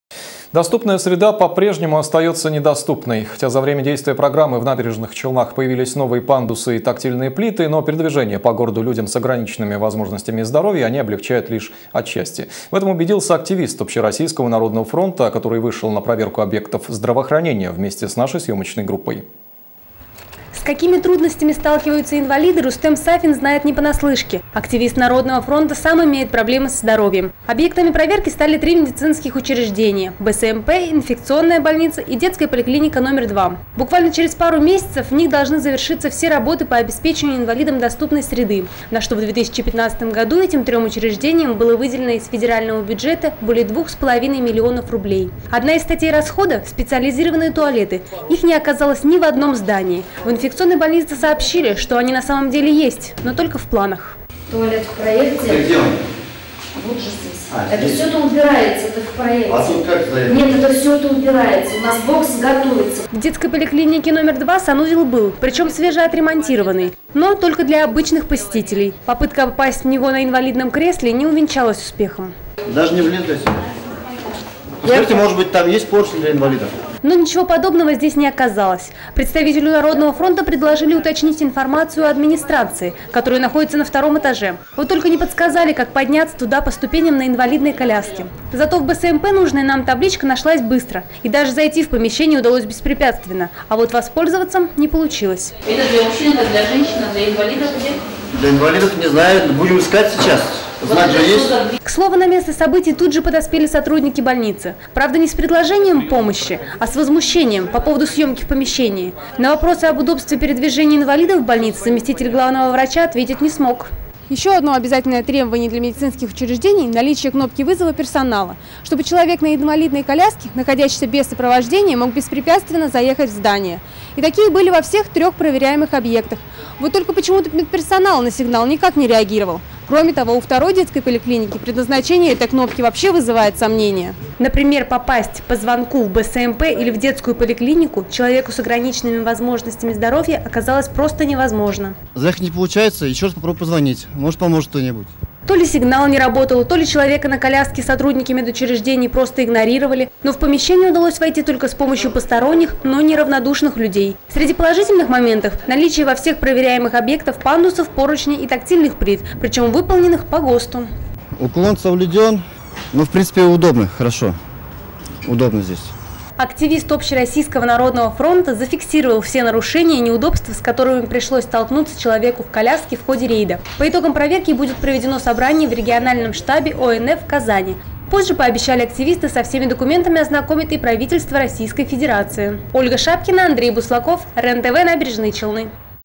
Доступность среды - пока в проекте
«Доступная среда» по-прежнему остаётся недоступной. Хотя за время действия программы в Набережных Челнах появились новые пандусы и тактильные плиты, передвижение по городу людям с ограниченными возможностями здоровья они облегчают лишь отчасти.